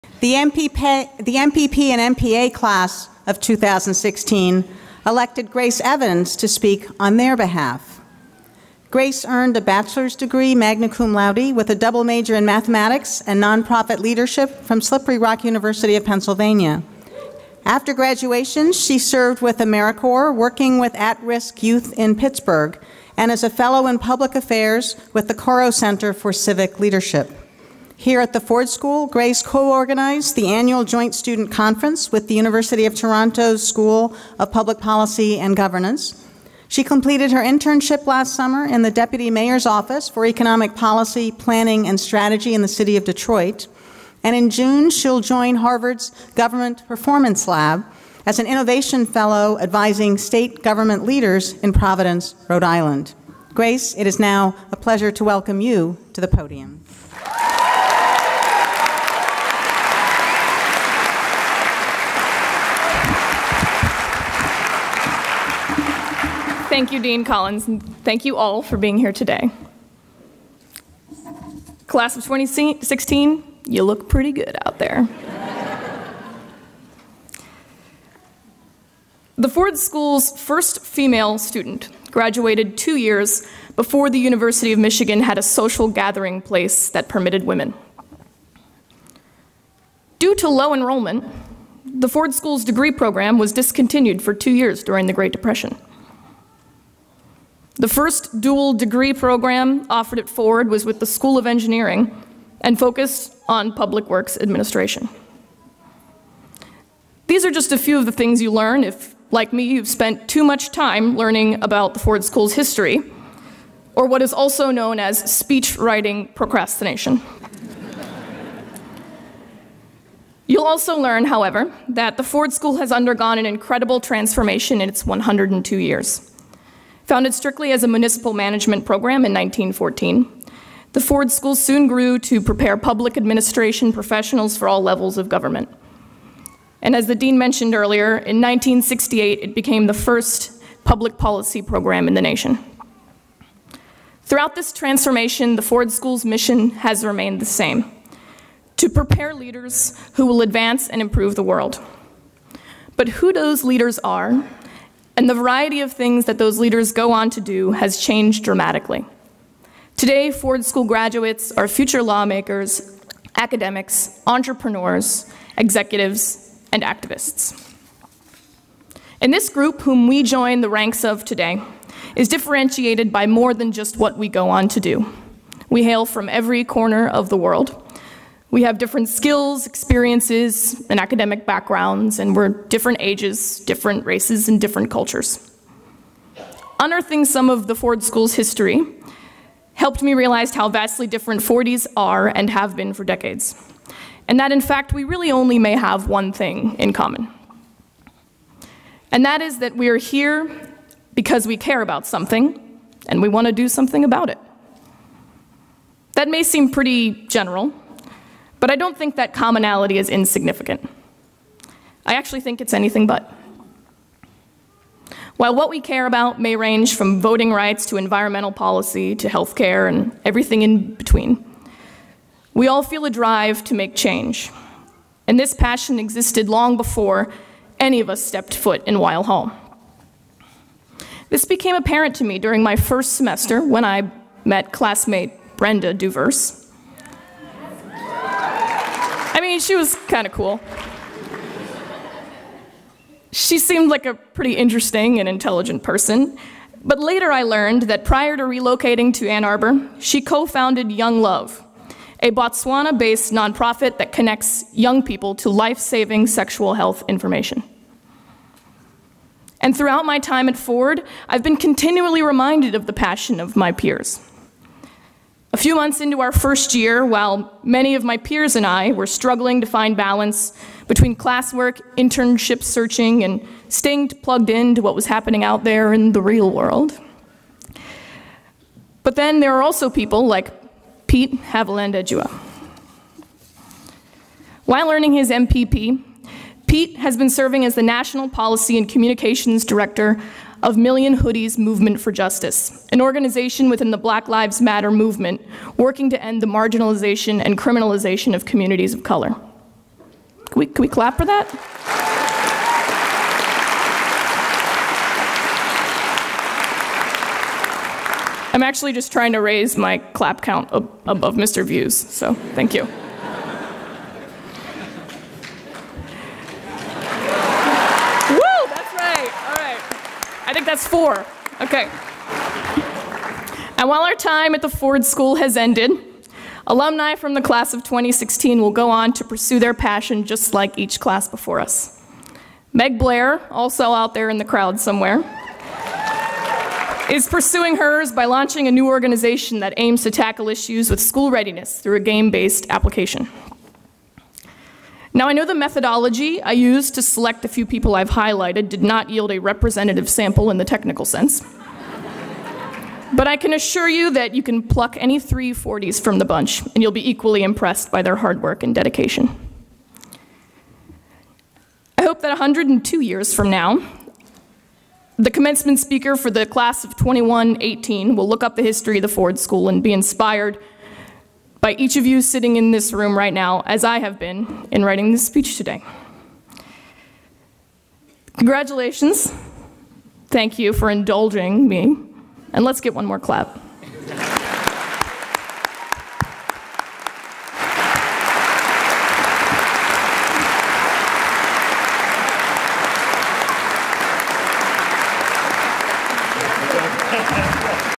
2016 Ford School Commencement - Grace Evans (MPP '16)
Grace Evans gives remarks on behalf of the master's students at the 2016 Ford School Commencement.